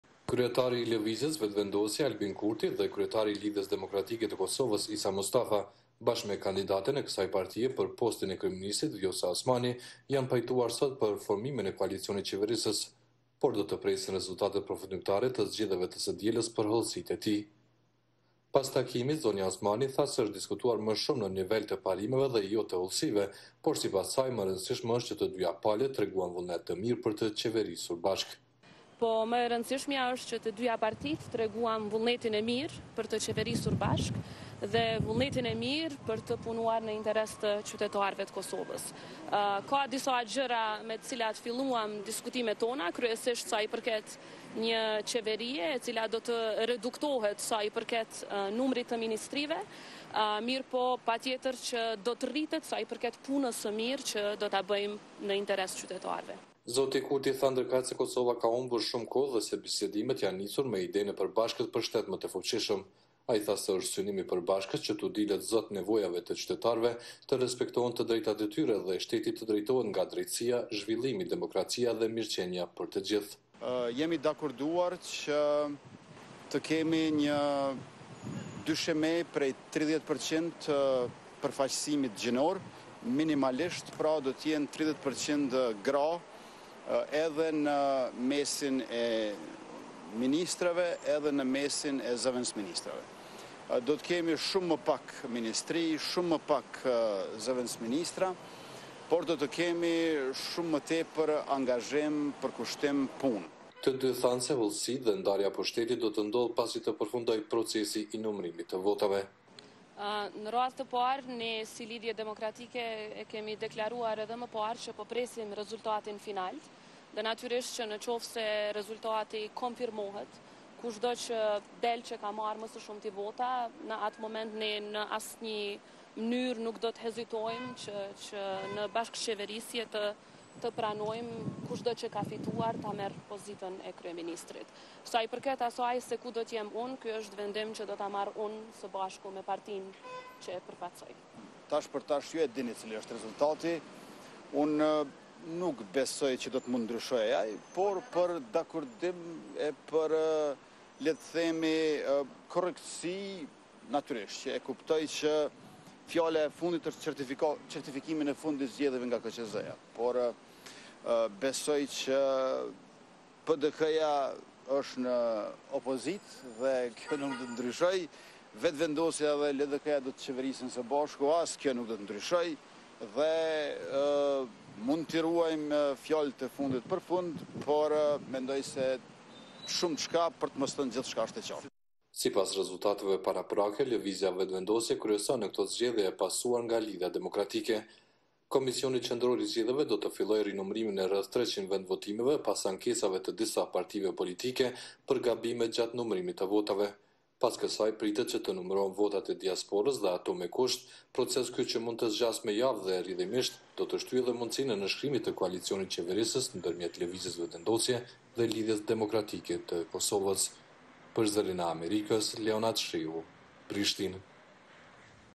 Vetëvendosje dhe LDK pajtohen për bashkëqeverisje
Udhëheqësit e lëvizjes Vetëvendosje dhe Lidhjes Demokratike të Kosovës u pajtuan sot për formimin e koalicionit qeverisës, por do të presin rezultatet përfundimtare të zgjedhjeve të së dielës për hollësitë e tij.